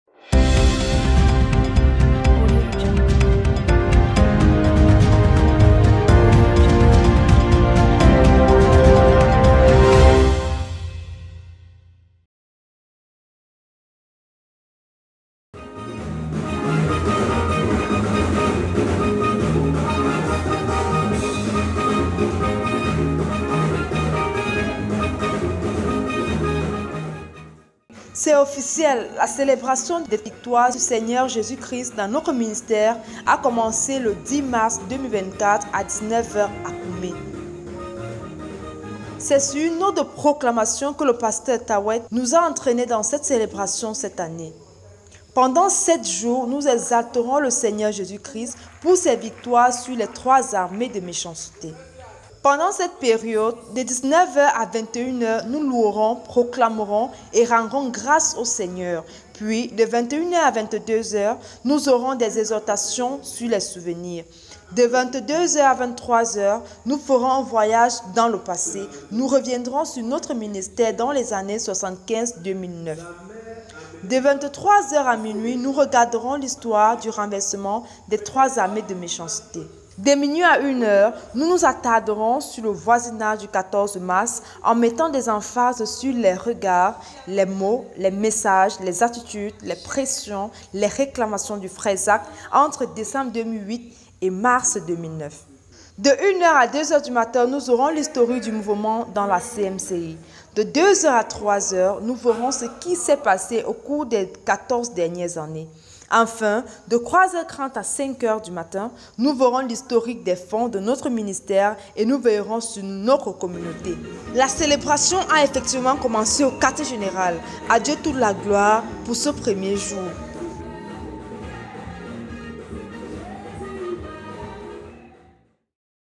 CELEBRATION | JOUR 1
Vous regardez un résumé de la journee de celebration a koume.